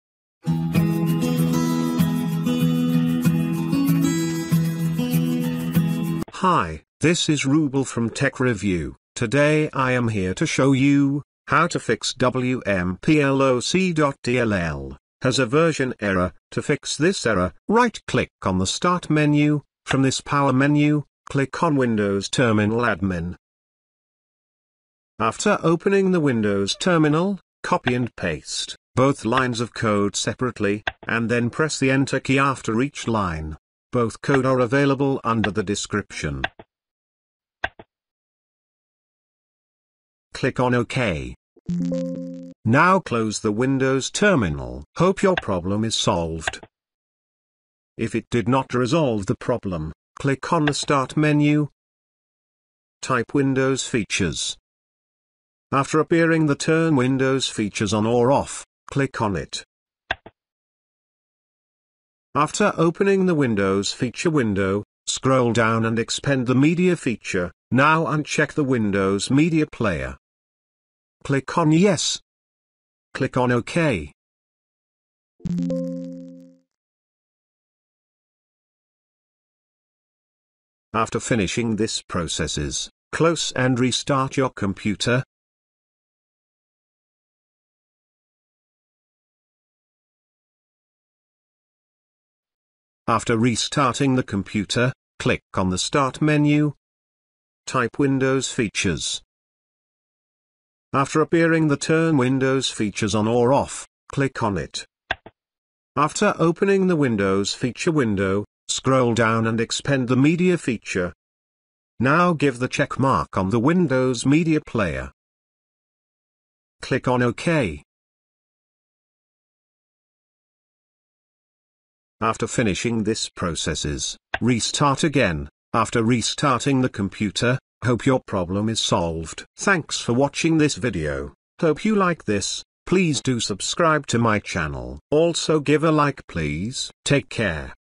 (Solved) How To Fix WMPLOC.DLL Has A Version Error
Windows has its own media player , which plays media quite well. This media player supports a number of well-known video and audio formats. Some people have experienced an issue when trying to open Windows Media Player in Windows 8.1 , showing an error that the wmploc.dll file has a version number of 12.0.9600.xxxxx where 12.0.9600.yyyyy was expected and Windows Media Player was not properly installed and must be reinstalled.

The version number may differ depending on the version of the media player.

This error is related to wmploc.dll. Thus, it can be fixed with the following methods.

Tech Review